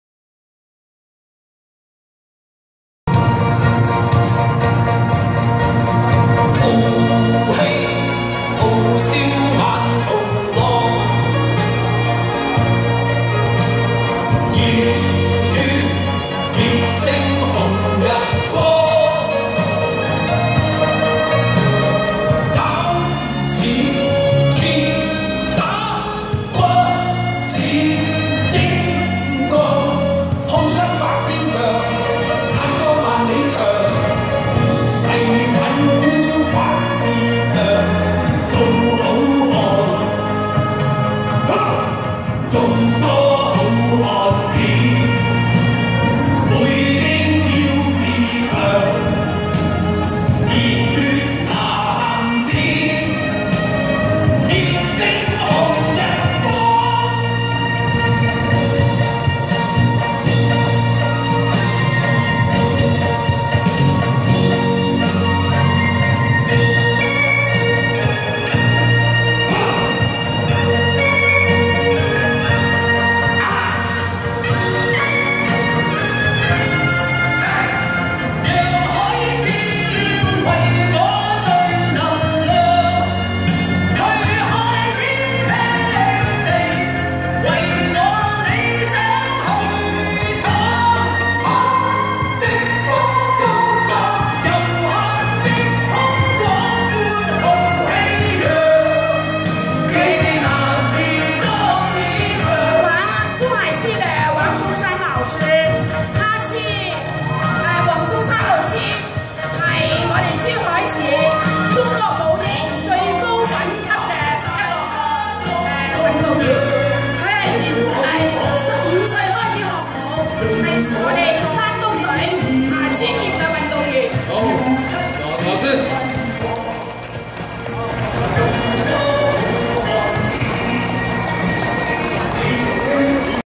珠海市王崇山老師 耍出 傳統螳螂拳風格
珠海市王崇山老師 應香港羅維忠師傅邀請參加慶祝回歸20周年活動 耍出 傳統螳螂拳風格